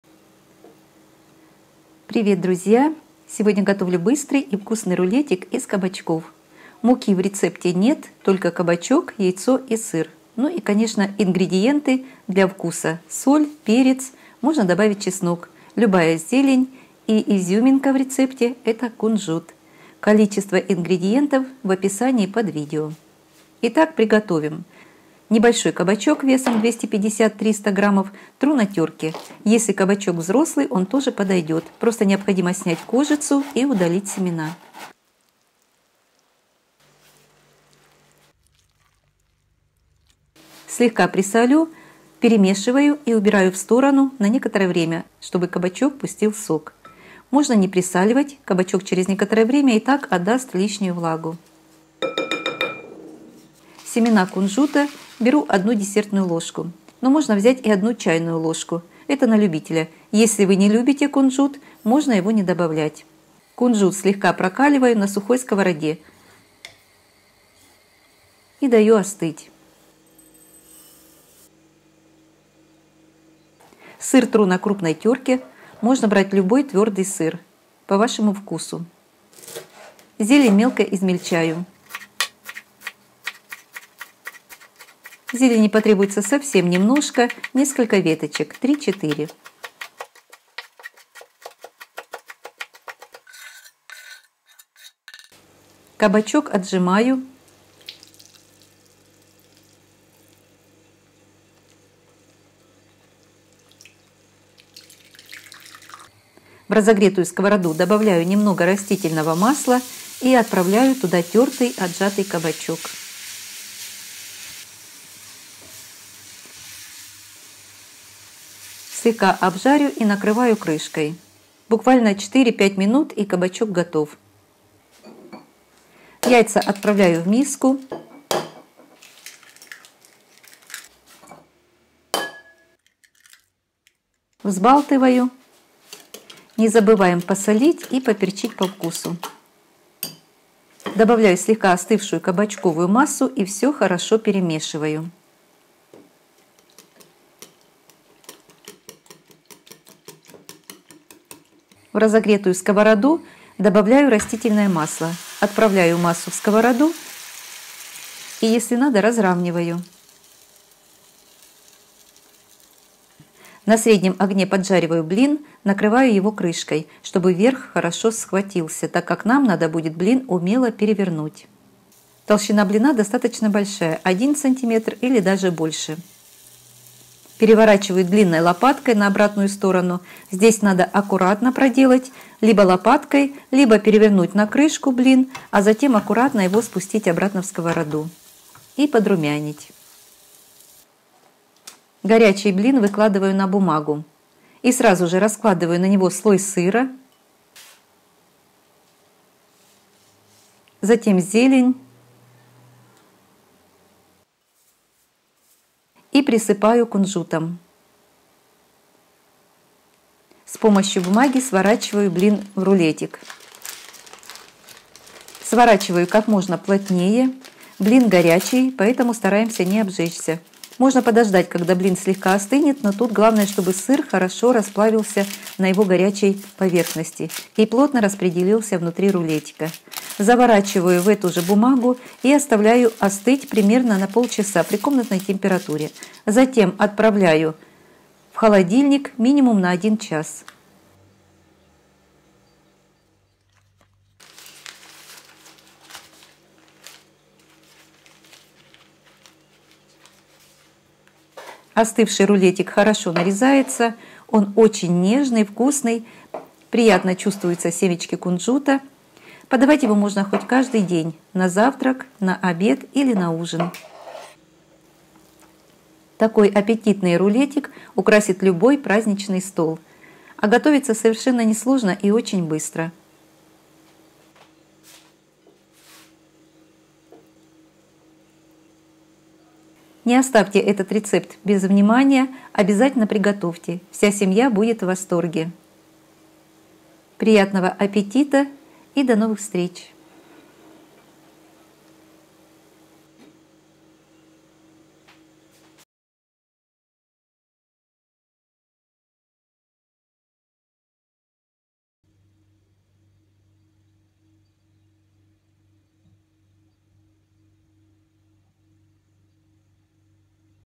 КАБАЧОК, ЯЙЦО, СЫР!!! Безумно вкусный кабачковый рулет. на сковороде! !!
Этот кабачковый рулет готовить можно каждый день.Он не надоесть. Все кто пробовал  в восторге от его нежного вкуса.
ИНГРЕДИЕНТЫ:
кабачок  250-350 гр .
яйца 3 шт
сыр  80-100 гр
кунжут 1 ч.л или 1 десертная ложка. Можно заменить орехами
3-4 веточки зелени любой(кинза, укроп, петрушка)
соль, перец по вкусу
растительное масло для обжарки

ягодный мусс на манке!!! легкий десерт!!!
творожно-малиновое суфле